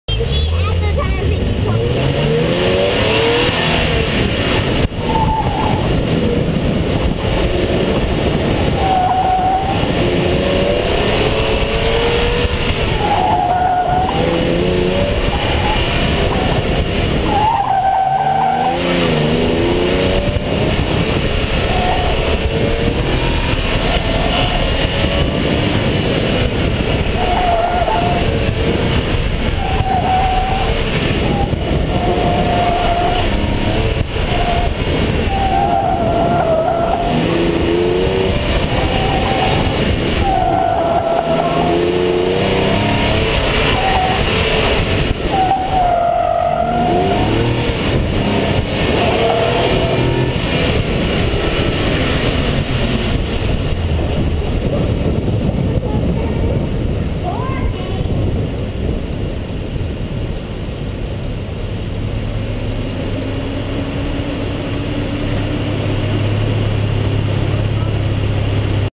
Philly SCCA Autocross Wells Fargo Clean Fastest Run 47.7s 8/24/13
My fastest clean run from yesterday: 47.7s. It's my first time running with Philly SCCA chapter. Another fantastic quick course from Andre. I ran only 1 second behind him which is pretty good I think.

PS: yes that's the voice of little kid. Yesterday, they actually had a kid say all the run times in heat 1 and 2. Which was cool.